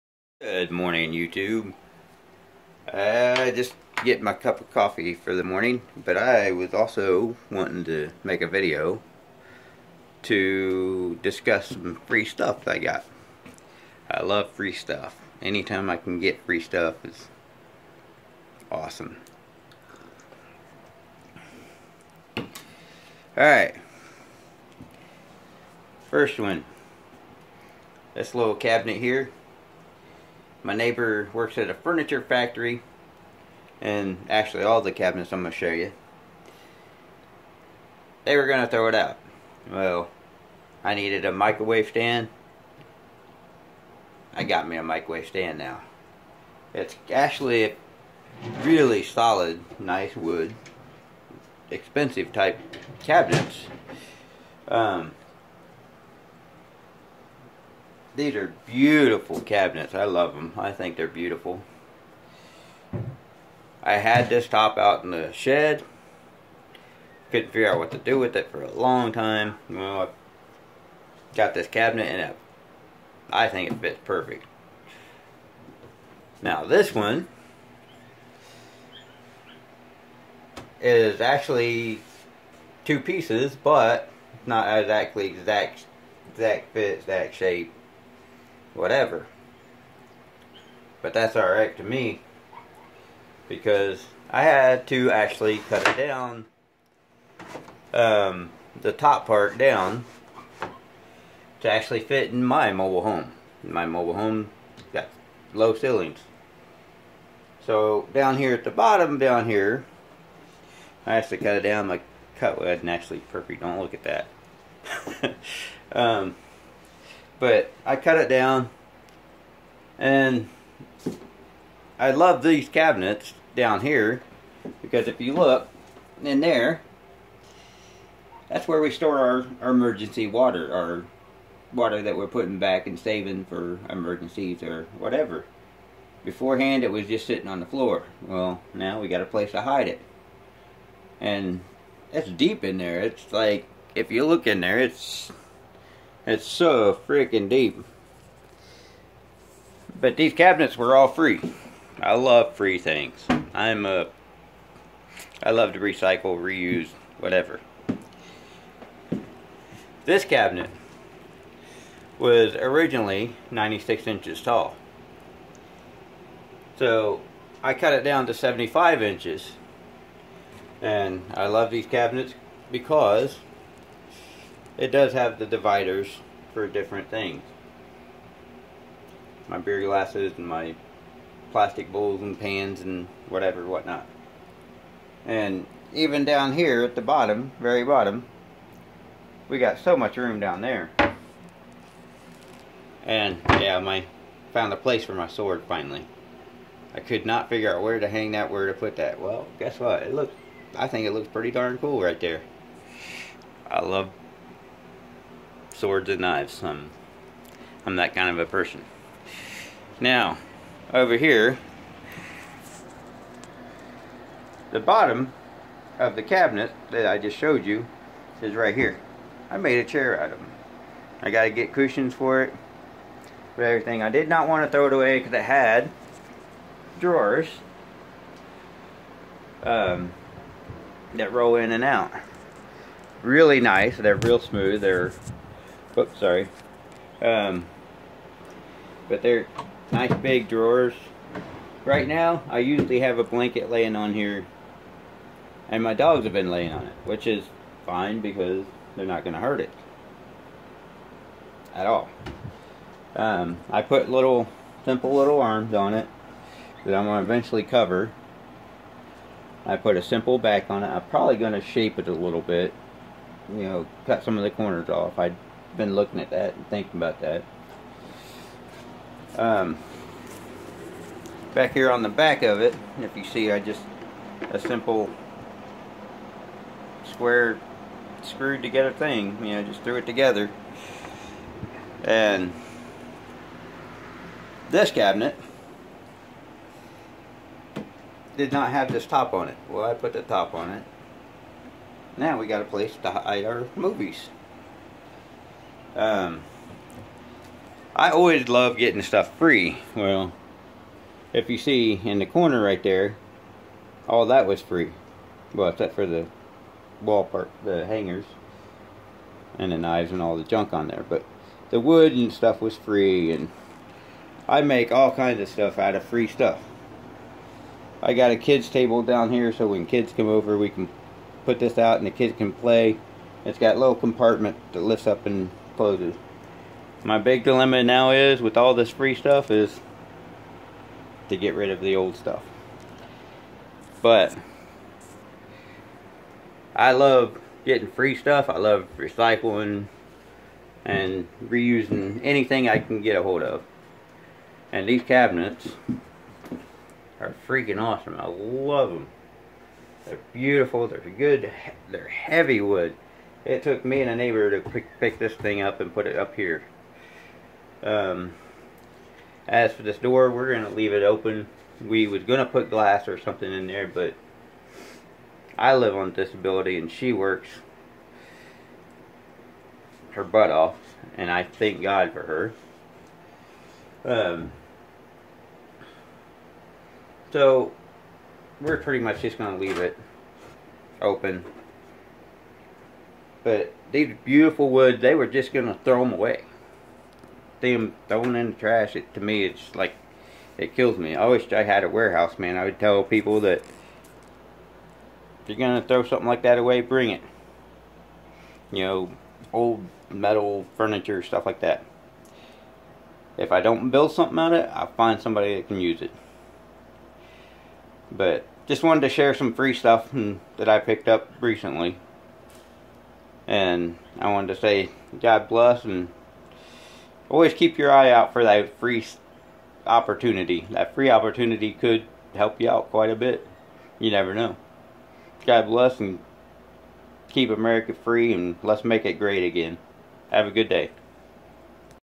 Good morning YouTube
Me talking about my free cabinet